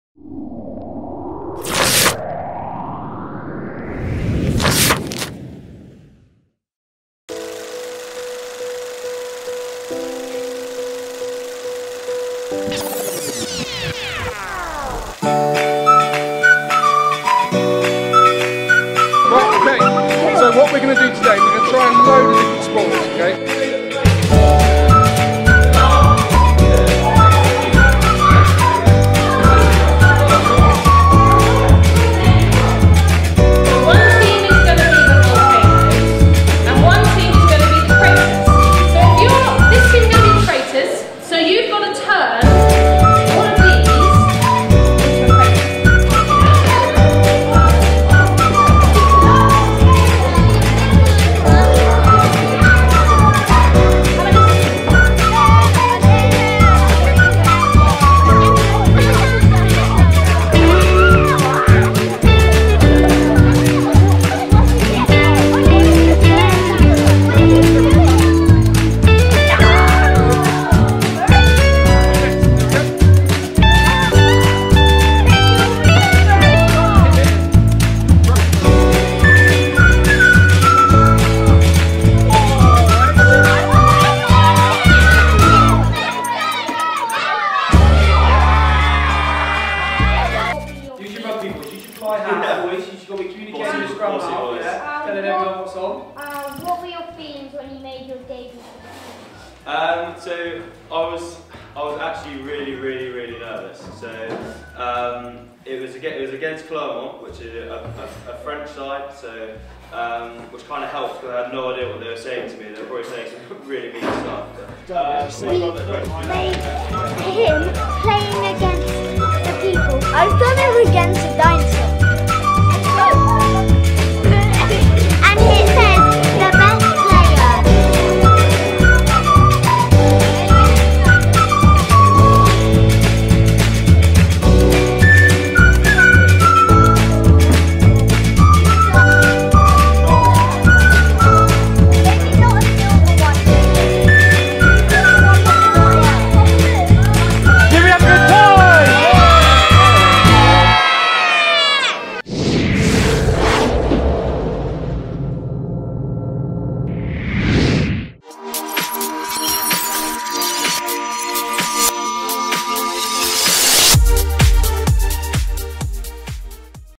The Great British Sports Show visits Denbury Primary School (Nov 2017)
The team from The Great British Sports Show visit Denbury Primary School - competition winners ahead of the Exeter Show on 3-4 February.  Every school child received an interactive multi-sport and physical literacy experience.